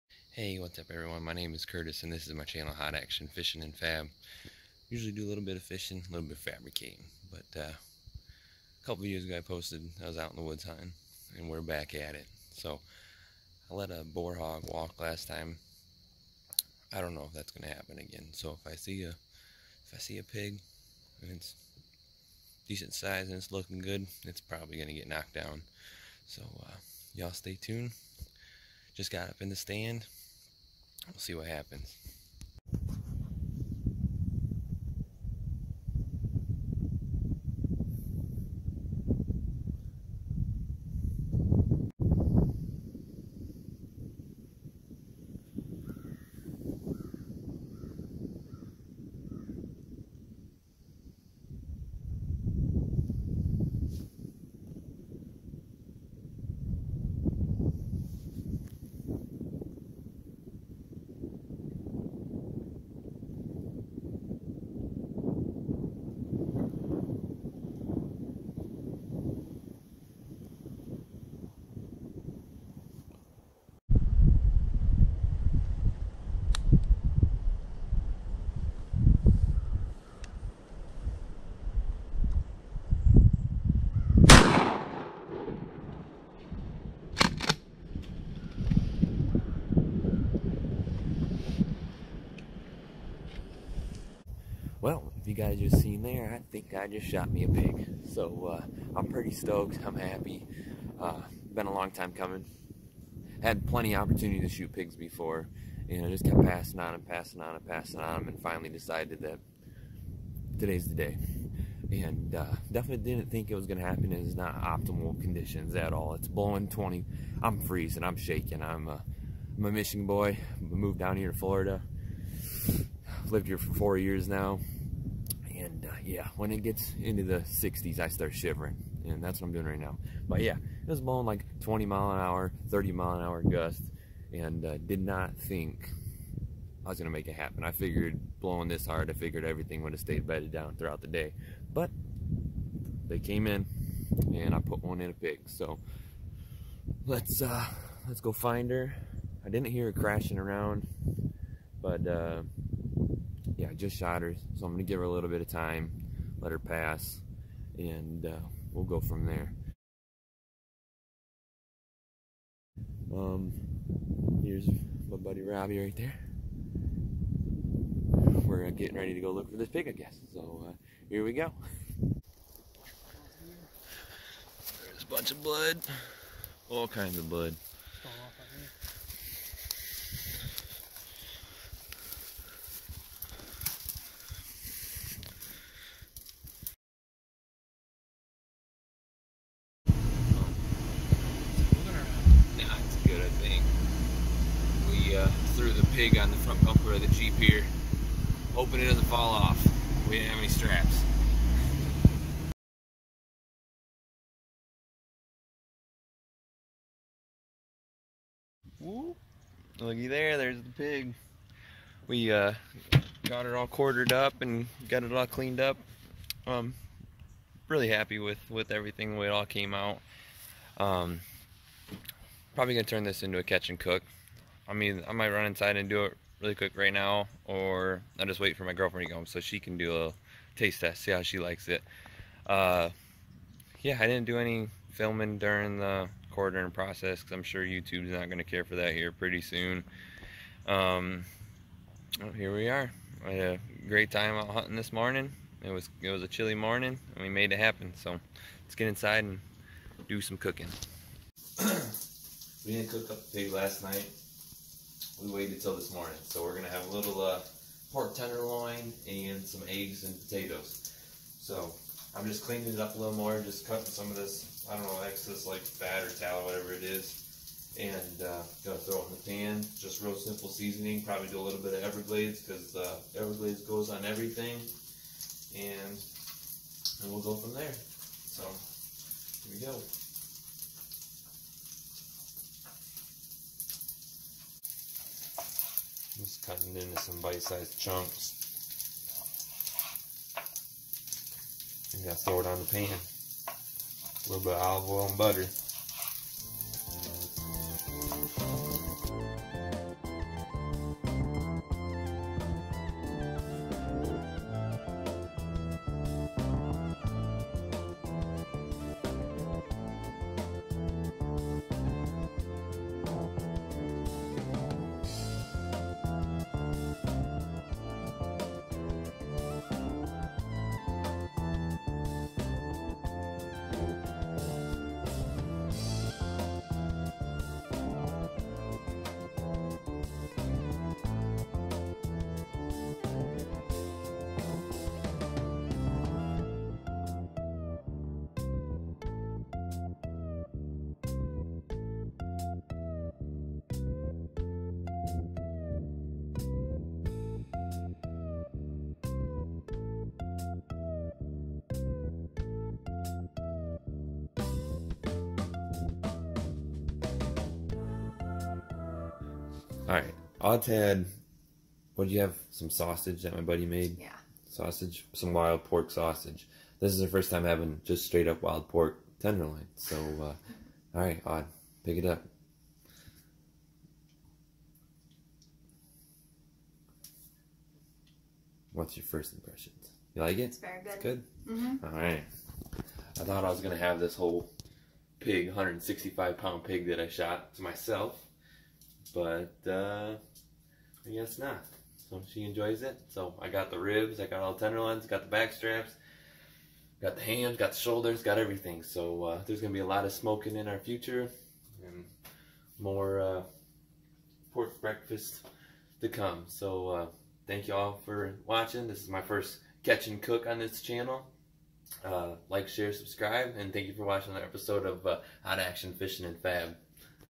Hog hunt South west Florida (Catch and Cook)
In this episode my buddy Robbie takes me out on a hog hunt and we were successful. Knocked down a big 165lb boar pig with my 12ga. At the end of the video we have a little taste test. It was great! Excited to get back out and do it again!